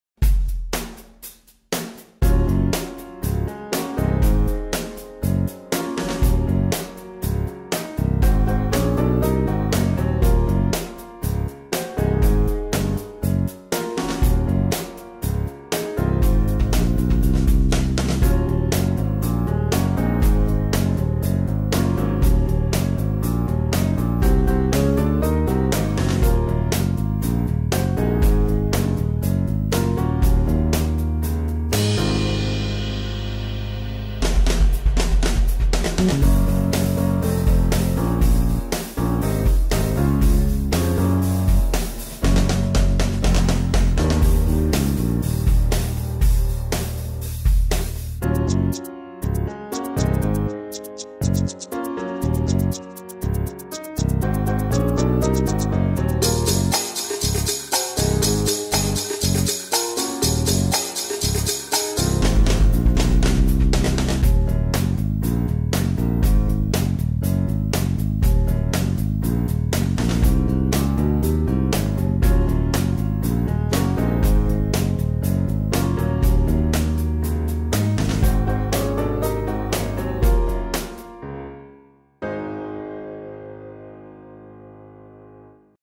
MANGANGAVA RECOLHENDO PÓLEN E NÉCTAR DA FLOR SETE-LÉGUAS
Sete-léguas - Planta trepadeira ideal para pérgolas, cercas e muros.
Bignonia rosa - Planta trepadora ideal para pérgolas, cercos y muros.
Nombre científico: Podranea ricasolana